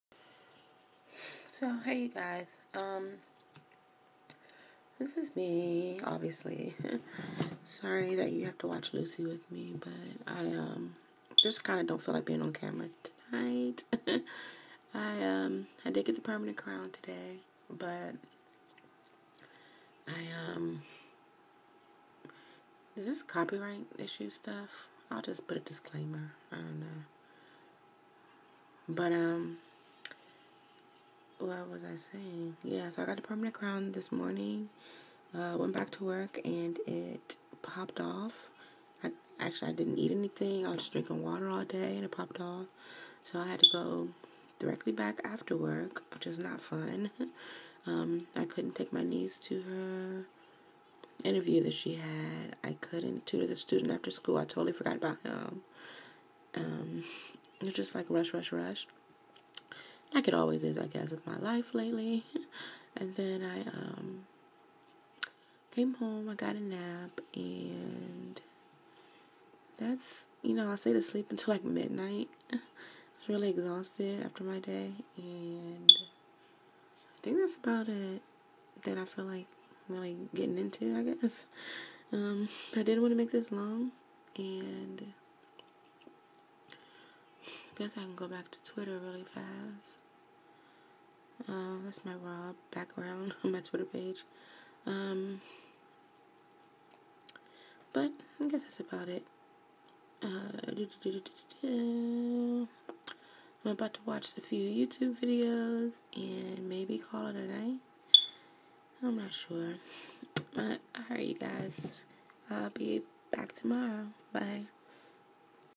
DV 49 - Lucy, Dental Woes, and Twitter
Just my day in a nutshell. Not feeling up to being on the other side of the lens.